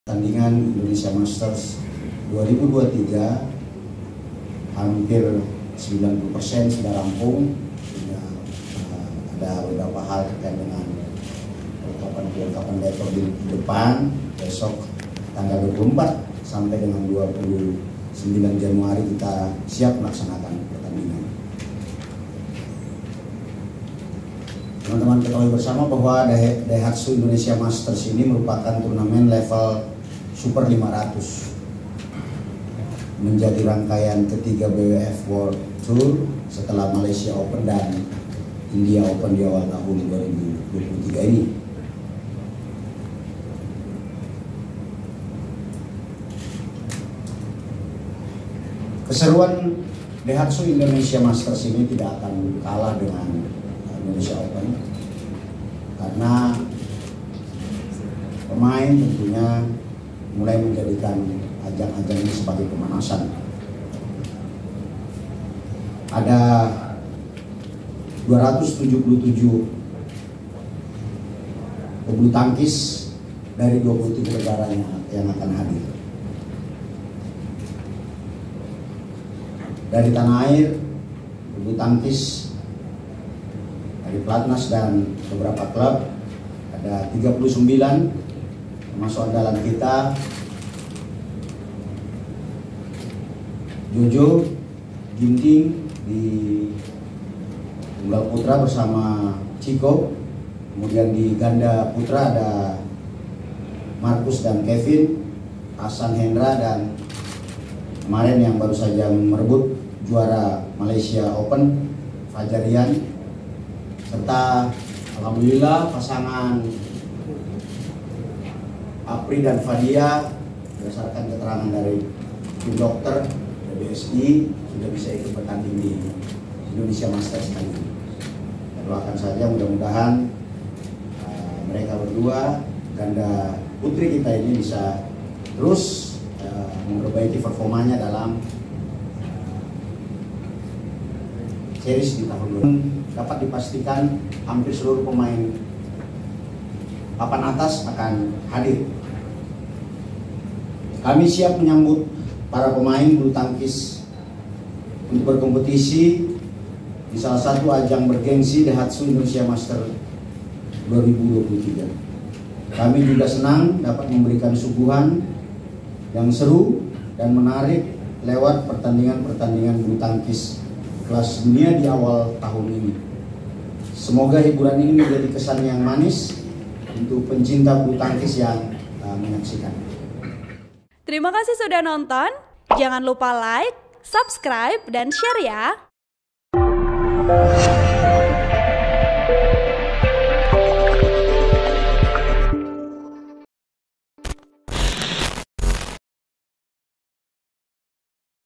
Apriyani dan Fadia Siap Tampil di Indonesia Masters 2023 di Istora Senayan Jakarta
Laporan Wartawan Tribunnews.com, Abdul Majid

TRIBUN-VIDEO.COM – Turnamen Bulutangkis Indonesia Masters akan terselenggara pada 24 -29 Januari 2023 di Istora Senayan, Jakarta.

Sekjen PP PBSI, Mohammad Fadil Imran mengatakan kesiapan penyelenggaraan Indonesia Masters pun sudah dalam tahap penyempurnaan.

Hal itu ia sampaikan dalam konferensi pers jelang gelaran Indonesia Masters di Istora Senayan, Jakarta, Senin (21/1/2023).

“Dapat kami laporkan saat ini persiapan Daihatsu Indonesia Masters 2023 sudah hampir rampung, 95 Istora sudah siap dan besok saat hari pertandingan semua sudah selesai,” kata Fadil

(Tribun-Video.com).